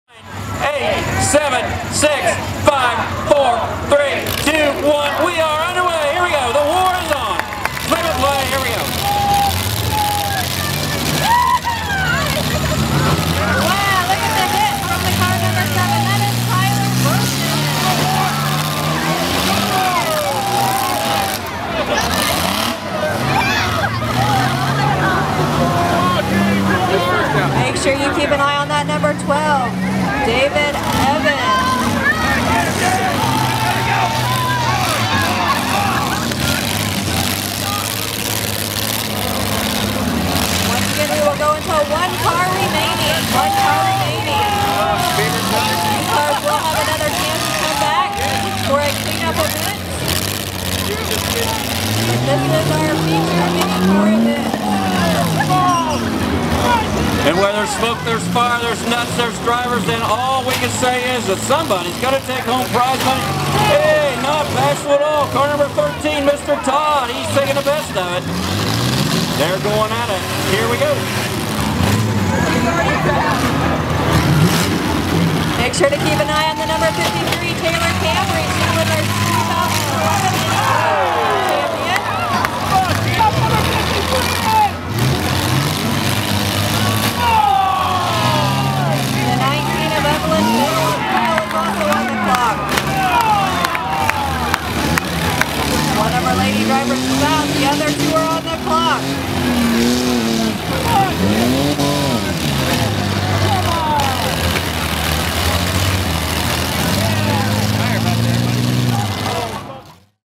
Firefighters' Fair: Demolition Derby
The Mini Cars try to disable one another during their division in the Demolition Derby at the Indian River County Firefighters' Fair. Staff video: Keith Carson.